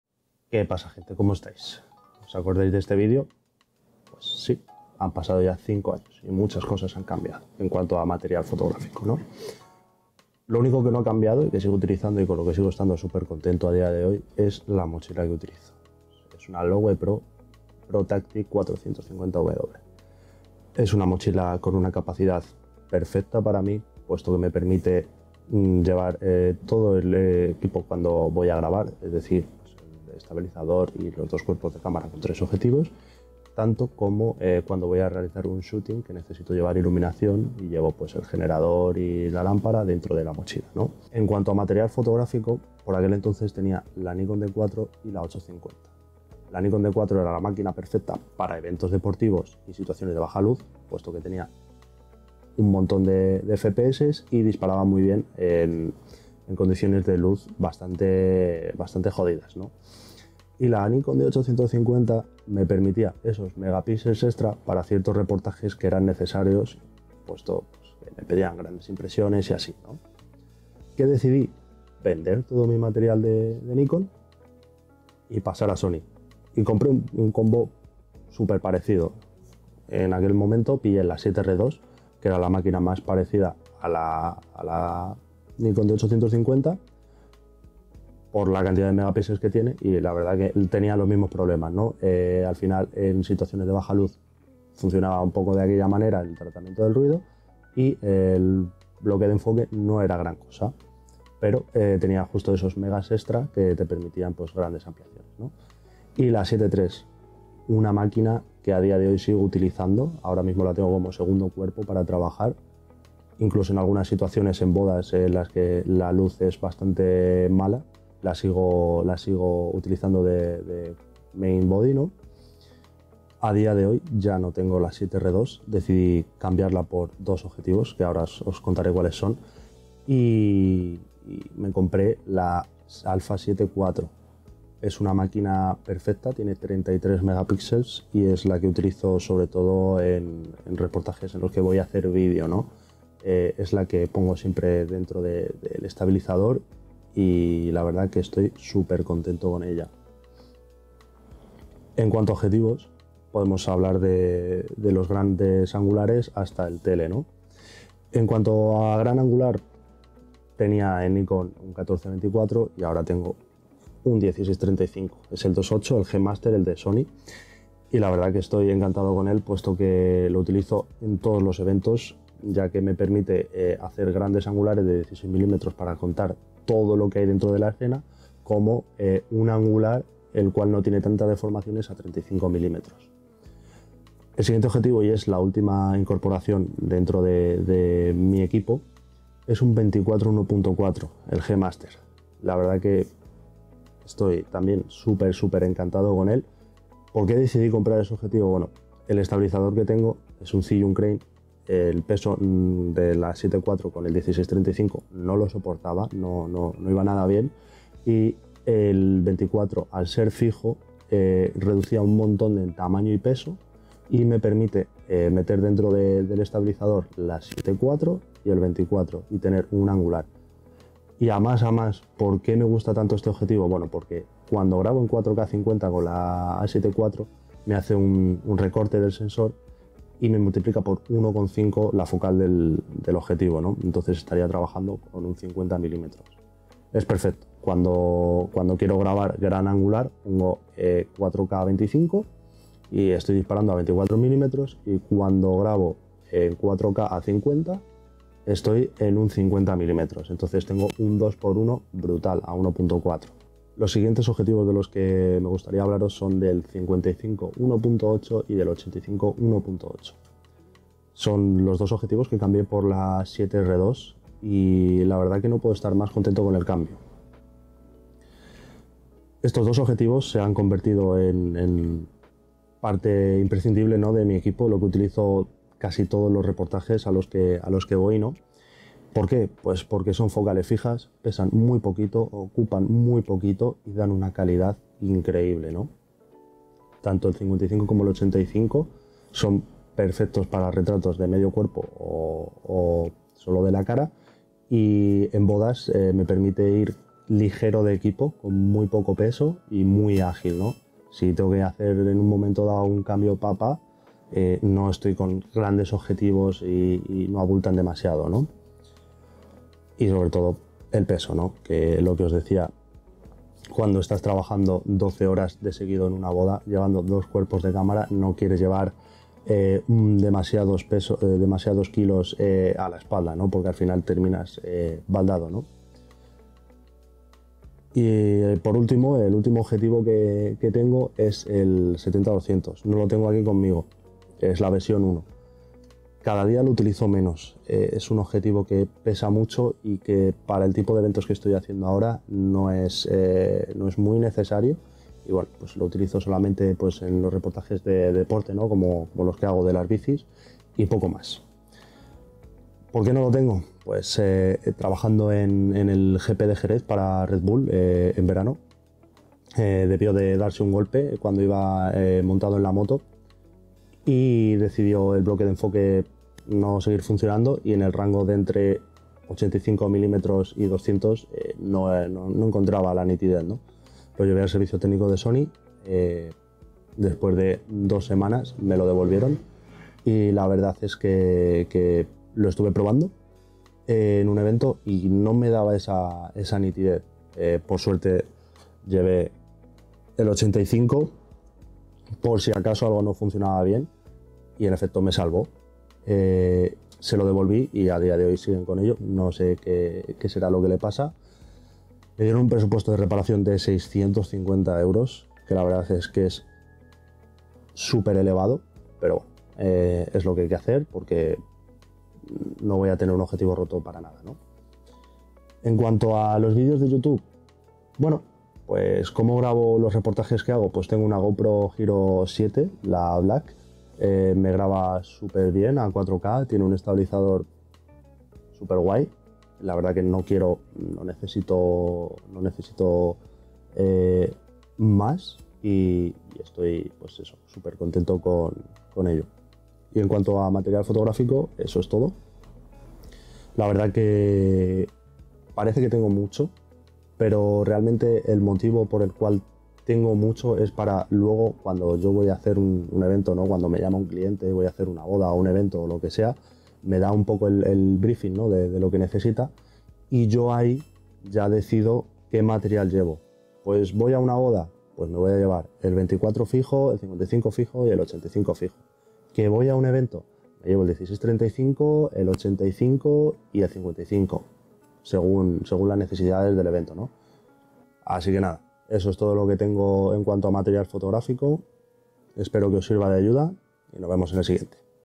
Mi equipo fotográfico 5 años después
En este video os comento los grandes cambios que ha habido en en mi equipo fotográfico 5 años después del primer video de este canal. Di el gran paso a Sony estoy más que contento con el material y la manera de trabajar.